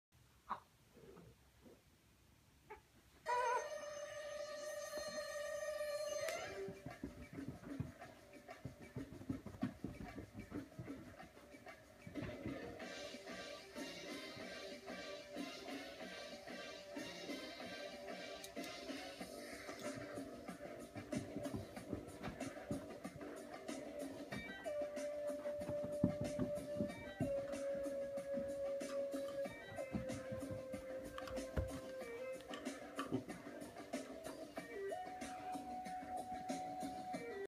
Mission impossible baby
Dad plays mission impossible toy grab with baby